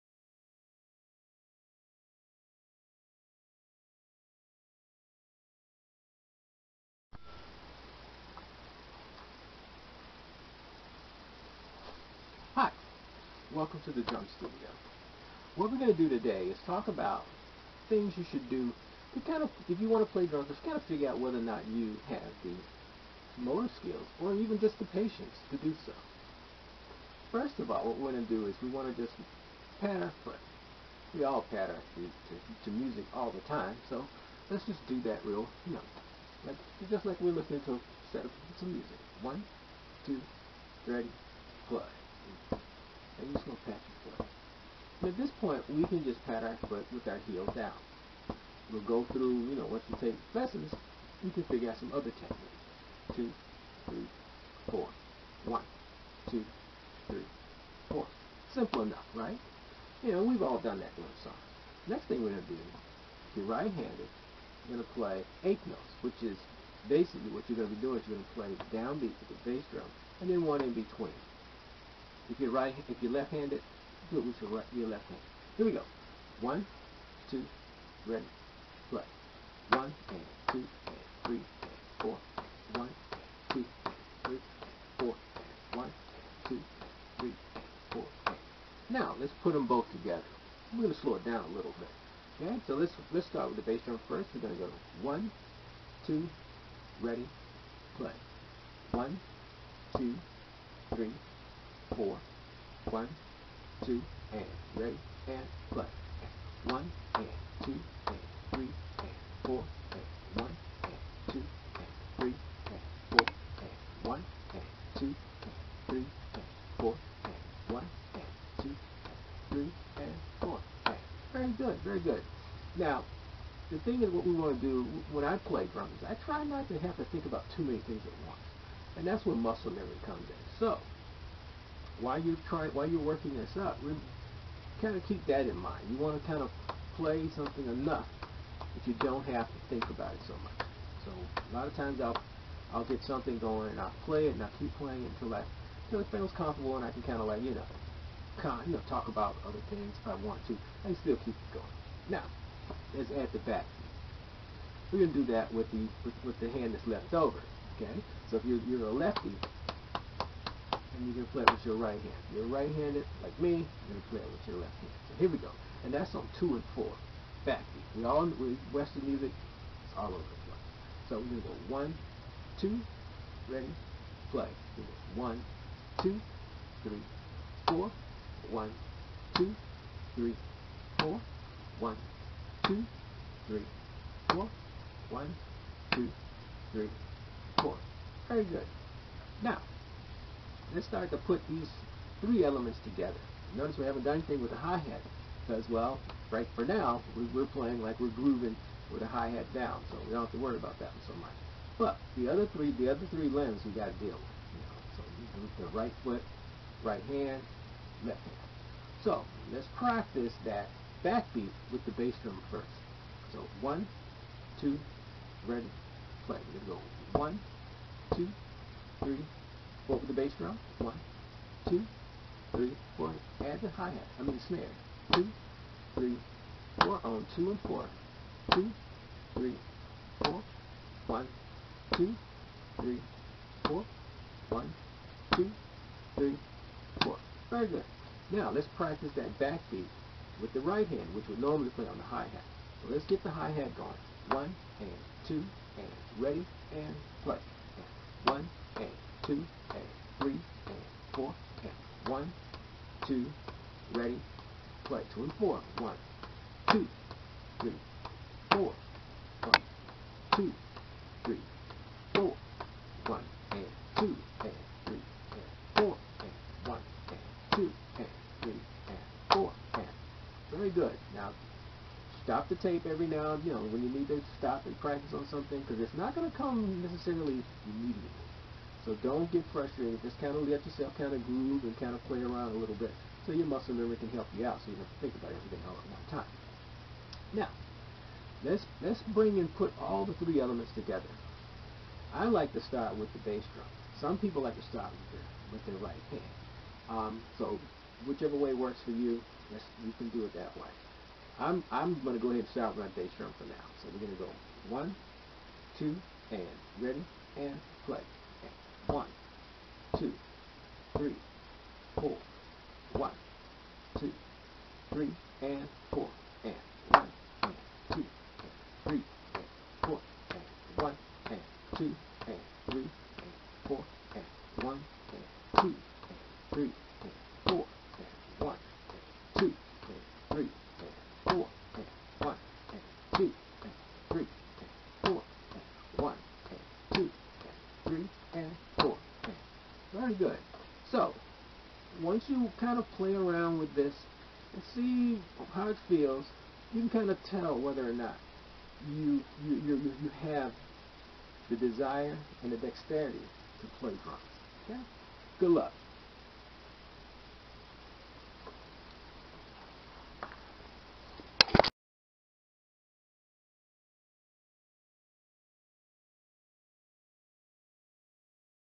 So You Want To Play Drum Set?
This Short Tutorial is for potential Drum Set students.  The Student should look at the video (play-a-long) and decide whether they have the desire and dexterity to play drum set.